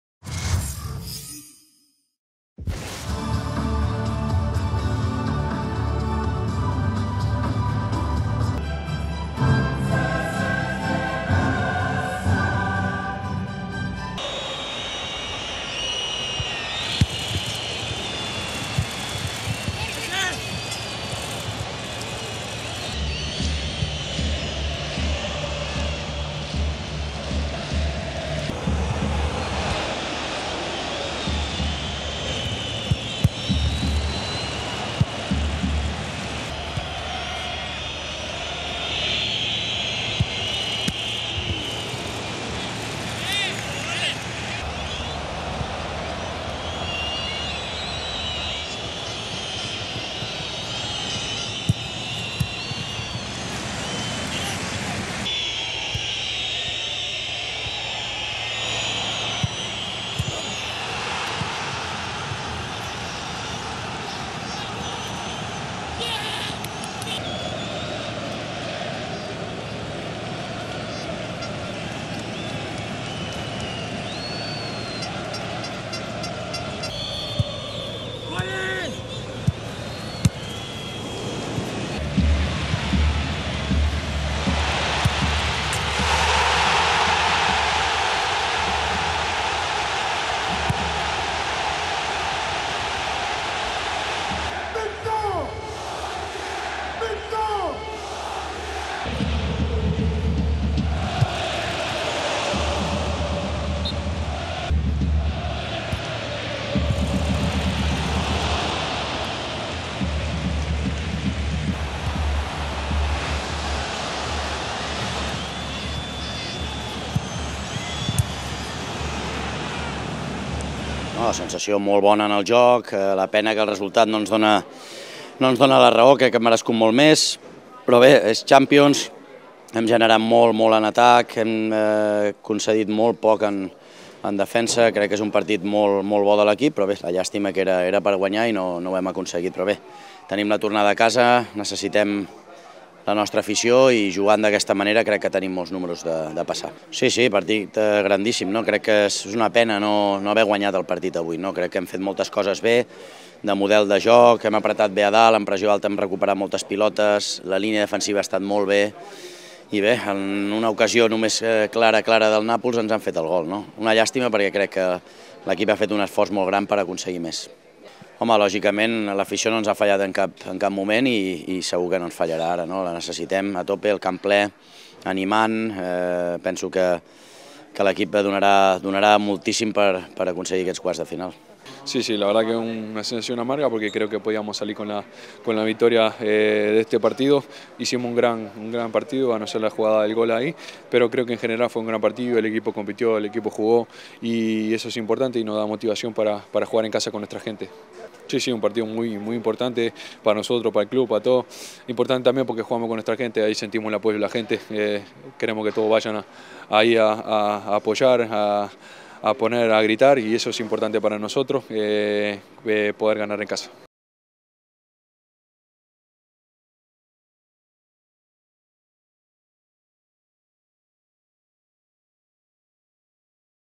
HIGHLIGHTS | NAPOLI 1 vs 1 FC BARCELONA | UEFA CHAMPIONS LEAGUE 2023/24
A1-1 draw away from home in the first leg of a Champions League knockout tie is never a bad result, but Barça will be coming back from Naples feeling that they deserved more than they actually got out of Wednesday’s clash.